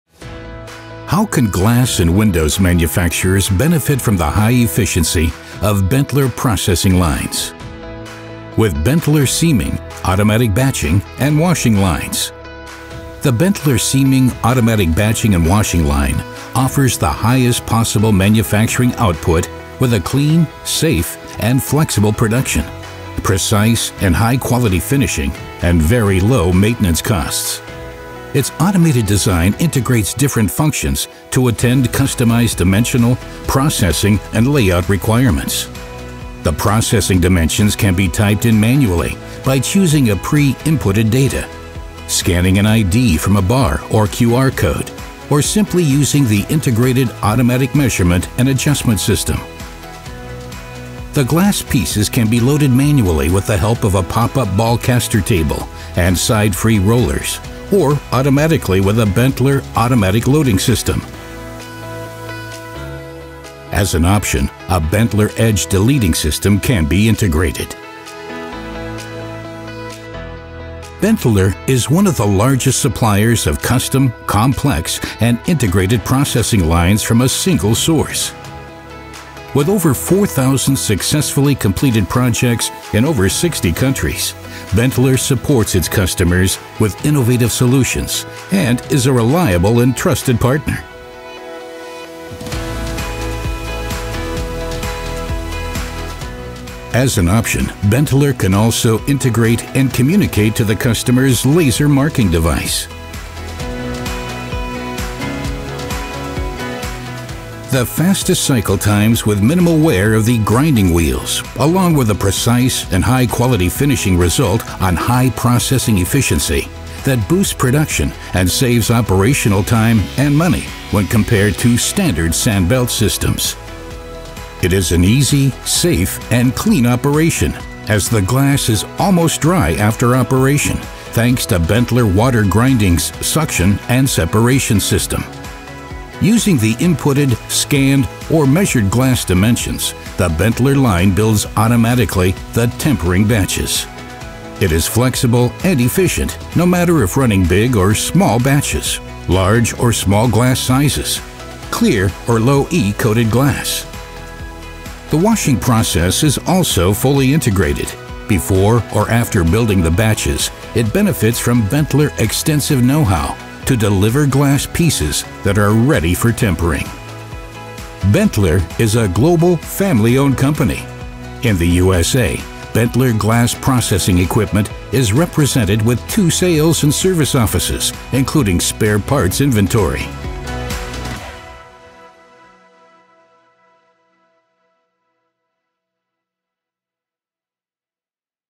Benteler Seaming Washing Line
Curious to find out how glass and windows manufacturers can benefit from the high efficiency of BENTELER processing lines? This video shows the BENTELER seaming, automatic batching and washing line.
Highest possible manufacturing output with a clean, safe and flexible production, precise and high-quality finishing and very low maintenance costs guaranteed